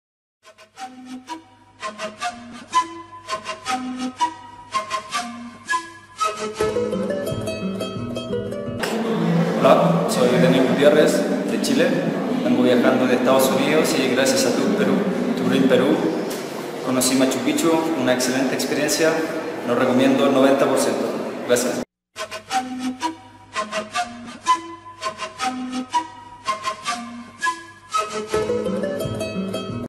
Joven Chileno Visita Machu Picchu
Chileno disfruta de una gran experiencia durante su visita a Machu Picchu. ven y visita Machu Picchu ahora contactanos a traves de nuestra pagina web